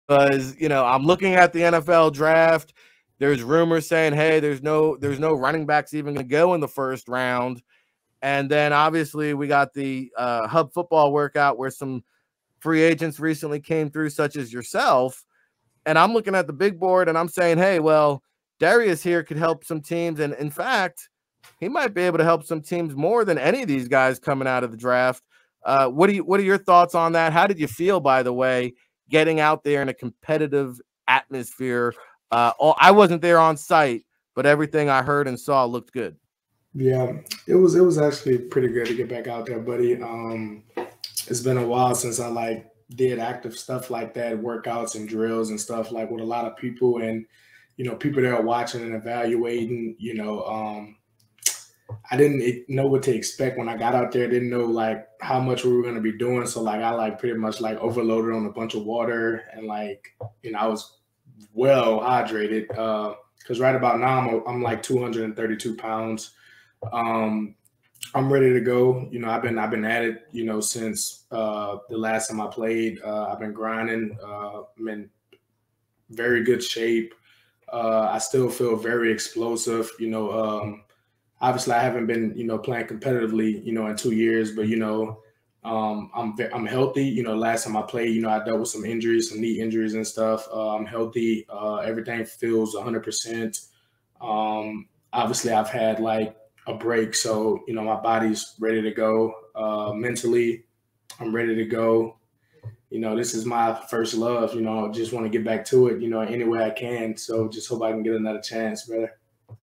All Access: Free Agent RB Derrius Guice Talks NFL Comeback
A second round pick of the Washington Commanders in 2018, free agent running back Derrius Guice joined Ric Serritella to discuss his latest NFL comeback attempt.

'All Access Football International Network'